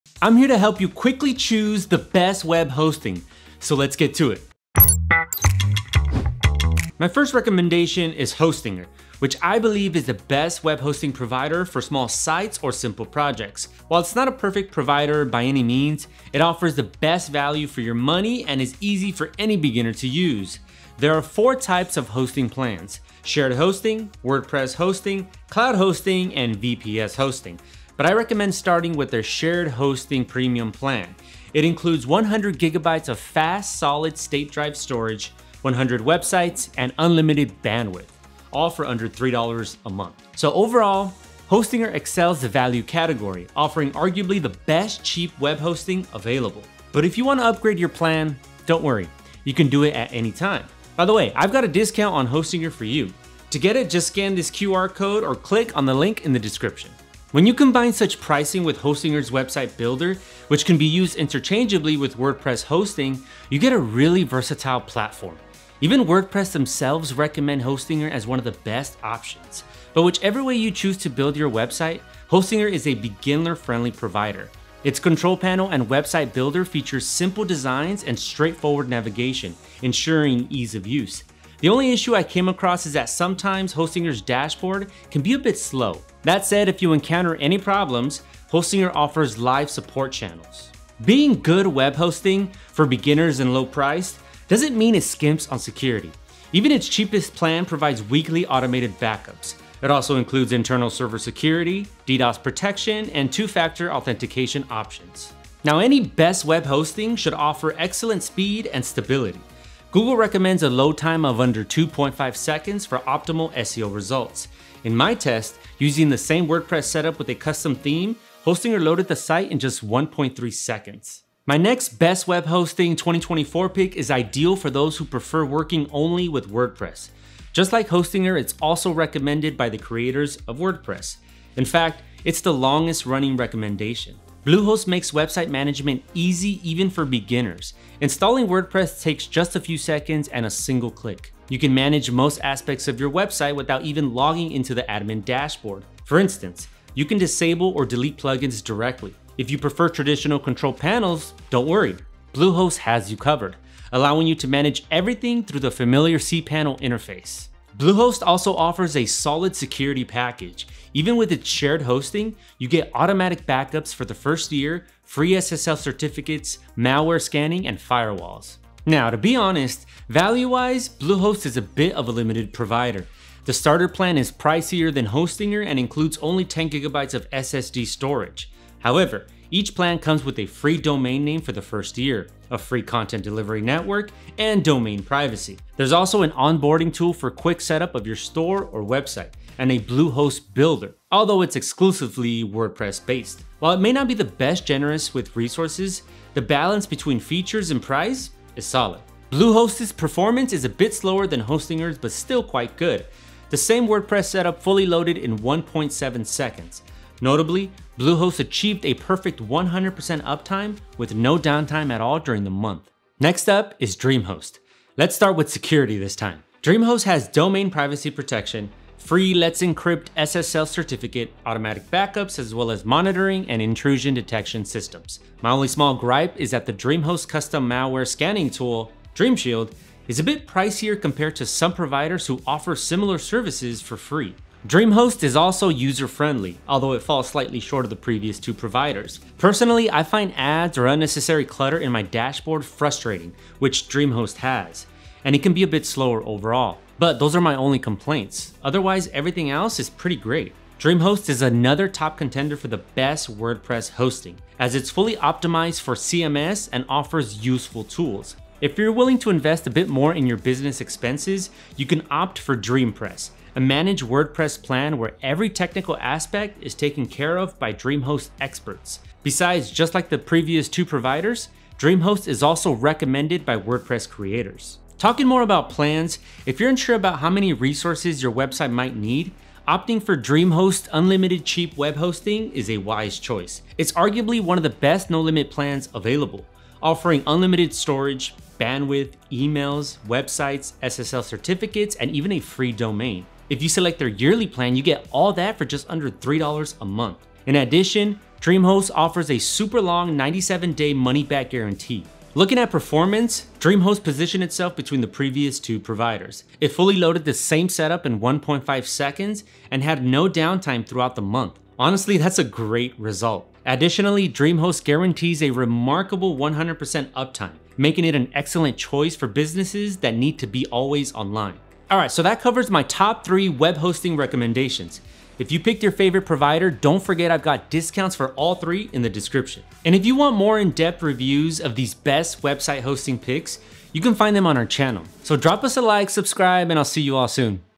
BEST Web Hosting in 5 MINUTES! 2025 guide
🧨 Get The BEST Hosting with a BIG discount right now! 🧨

I’m here to help you choose the best web hosting quickly, so let’s get to it!

Hostinger

My first recommendation is Hostinger, which I believe is the best web hosting provider for small sites or simple projects. While it's not a perfect provider by any means, it offers the best value for your money and is easy for any beginner to use.  There are 4 types of hosting plans: shared hosting, WordPress hosting, Cloud hosting, and VPS hosting. But I recommend starting with their shared hosting premium plan. It includes 100 GB of fast Solid State Drive storage, 100 websites, and unlimited bandwidth—all for under $3 a month. So overall, Hostinger excels in the value category, offering arguably the best cheap web hosting available. When you combine such pricing with Hostinger's Website Builder, which can be used interchangeably with WordPress hosting, you get a really versatile platform. Even WordPress themselves recommend Hostinger as one of the best options.

Bluehost

Just like Hostinger, Nluehost is also recommended by the creators of WordPress. In fact, it’s the longest-running recommendation. Bluehost makes website management easy, even for beginners. Installing WordPress takes just a few seconds and a single click. You can manage most aspects of your website without even logging into the admin dashboard; for instance, you can disable or delete plugins directly. If you prefer traditional control panels, don't worry—Bluehost has you covered, allowing you to manage everything through the familiar cPanel interface. Bluehost also offers a solid security package. Even with its shared hosting, you get automatic backups for the first year, free SSL certificates, malware scanning, and firewalls. Now, to be honest, value-wise, Bluehost is a bit limited provider. The starter plan is pricier than Hostinger and includes only 10GB of SSD storage. However, each plan comes with a free domain name for the first year, a free content delivery network, and domain privacy.

DreamHost

DreamHost is another top contender for the best wordpress hosting, as it's fully optimized for this CMS and offers useful tools. If you're willing to invest a bit more in your business expenses, you can opt for DreamPress, a managed WordPress plan where every technical aspect is taken care of by DreamHost experts. Besides, just like the previous 2 providers, Dreamhost is also recommended by WordPress creators.DreamHost is also user-friendly, although it falls slightly short of the previous two providers. Personally, I find ads or unnecessary clutter in my dashboard frustrating, which DreamHost has, and it can be a bit slower overall. But, those are my only complaints; otherwise, everything else is pretty great.

00:00 Intro
0:06 Hostinger - best cheap web hosting
1:30 Hostinger security and features
2:01 Bluehost - great pick for WordPress users
2:37 Bluehost pricing and plans
3:33 Dreamhost - great pick for business owners
4:18 Dreamhost pricing and plans
5:22 Conclusion

The footage is provided by Envato